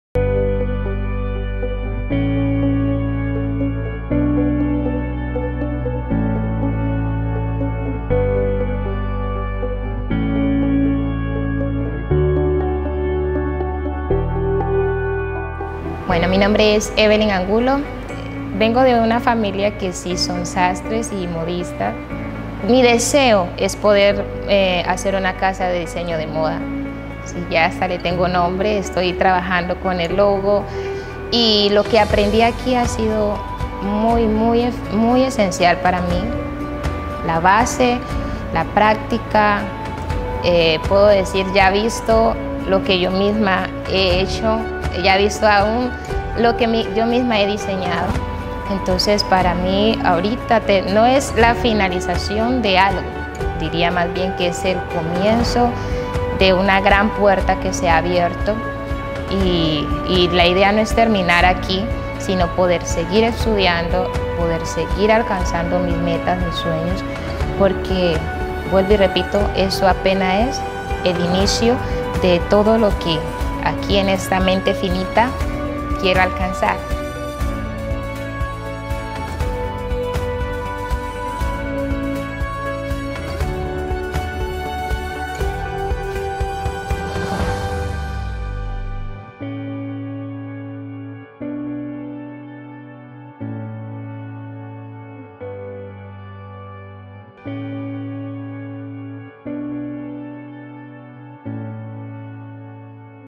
Mujeres Confeccionistas Emprendedoras GYE
Gracias a nuestro programa de Mujeres Confeccionistas, muchas mujeres se han empoderado de sus vidas y han logrado sus metas.